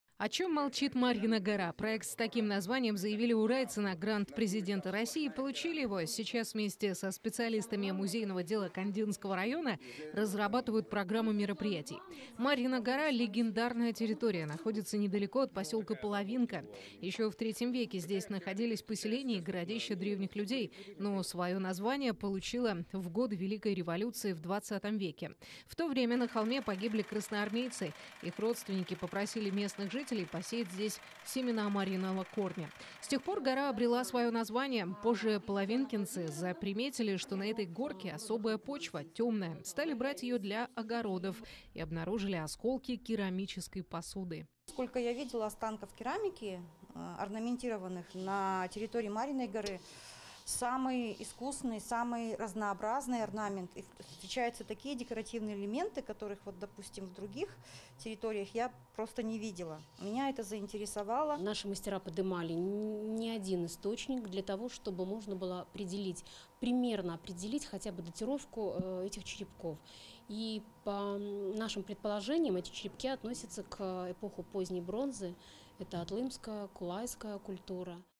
Урайцы выиграли президентский грант, чтобы рассказать историю Марьиной горы
«О чём молчит Марьина гора» - проект с таким названием заявили урайцы на грант президента России и получили его. Сейчас вместе со специалистами музейного дела Кондинского района разрабатывают программу мероприятий.